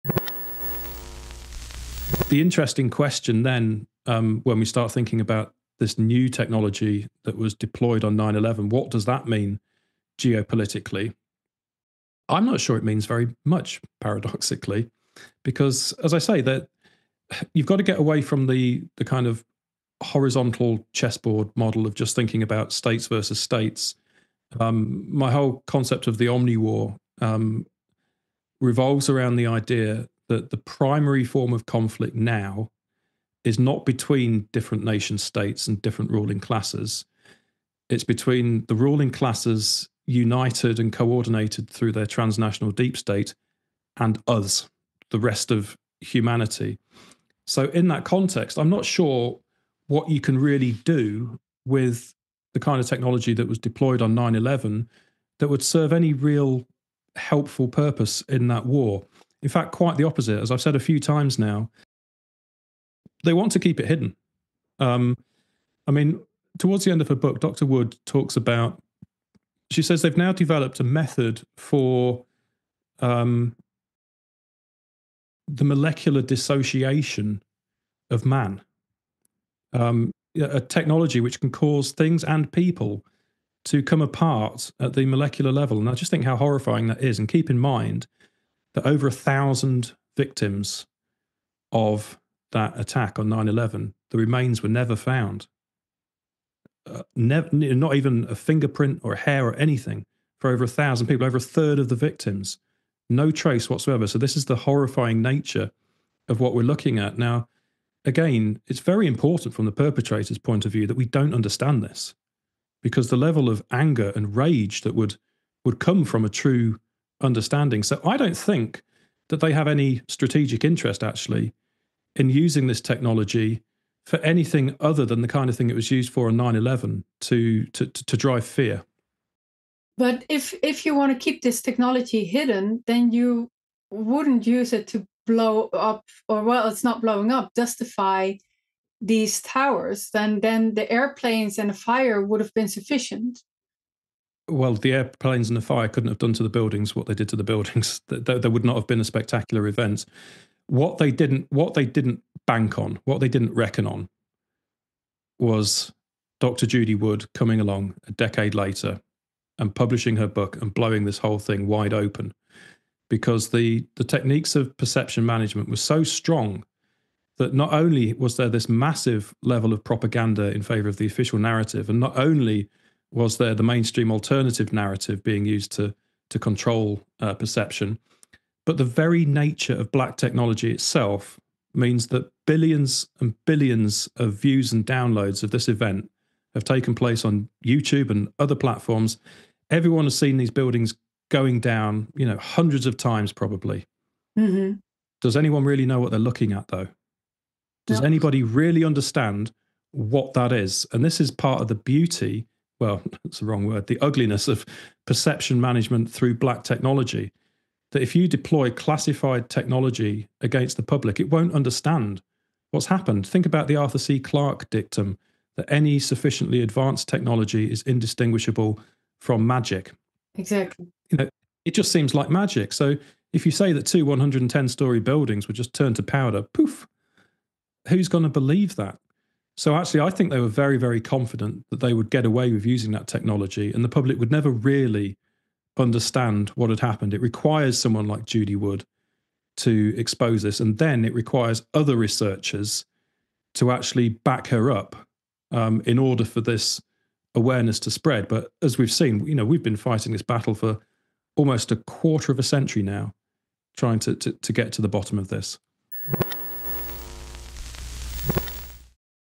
The 9/11 Perpetrators’ Horrifying Tech Can Cause Things & People to Come Apart at Molecular Level
The Implications of Dr. Judy Wood’s Work with David A. Hughes

In 2010, Dr. Judy Wood published her groundbreaking book, Where Did the Towers Go? Evidence of Directed Free-energy Technology on 9/11, in which she presented ample forensic evidence indicating the deployment on 9/11 of some technology unknown to the general public. Wood’s work has not received the attention it deserves. International relations scholar David A. Hughes, PhD is trying to change that by publishing a series of articles in defense of Wood. In this interview, Hughes returns to the Solari Report to join me in discussing the geopolitical and other major implications of Dr. Wood’s work on 9/11’s invisible weaponry.

Wood is a former mechanical engineering professor who received her PhD in materials engineering science from Virginia Tech. In the 500 pages of her book, which includes numerous high-quality images, she argues that “Empirical evidence is the truth that theory must mimic” and shows that what we see in the pictures does not match the explanatory narratives presented to us—neither the “official reality” narrative of airplanes causing the collapse nor the alternative theories of a “pancake collapse” caused by nanothermite or mini-nukes. For example, hardly any debris—from two towers weighing half a million tons each—was visible at Ground Zero just moments after the event. According to Wood, the towers did not so much collapse as appear to turn into dust mid-air.